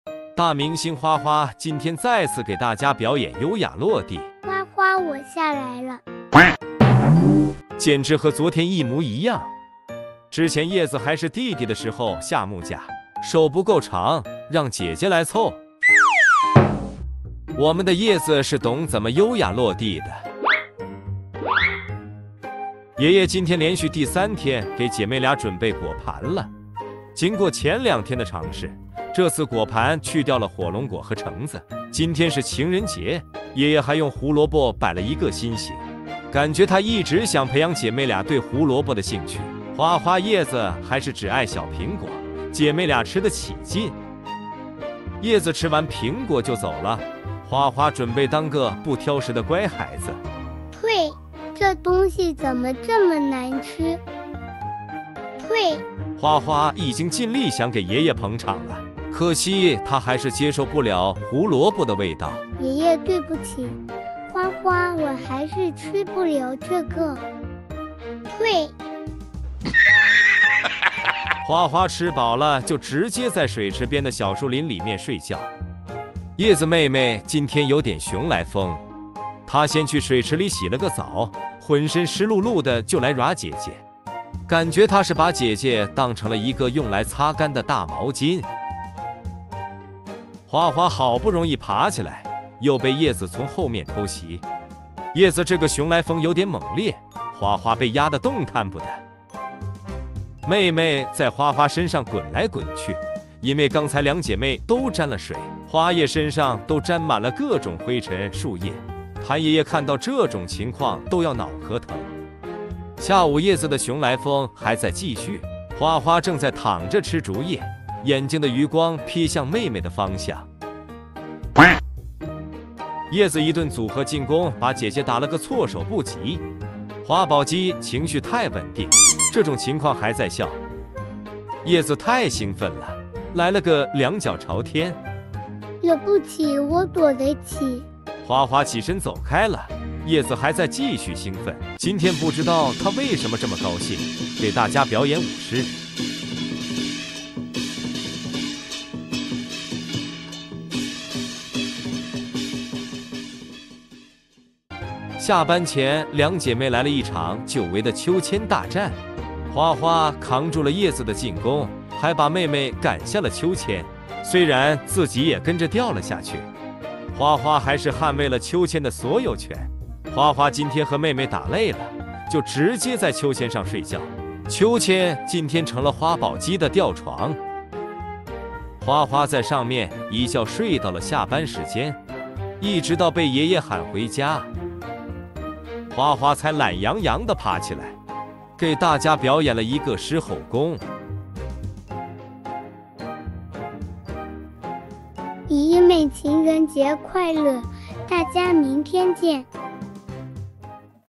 2.14爺爺用胡蘿蔔擺情人節愛心，結果花花吃了吐，葉子熊來瘋一直跟姊姊打鬧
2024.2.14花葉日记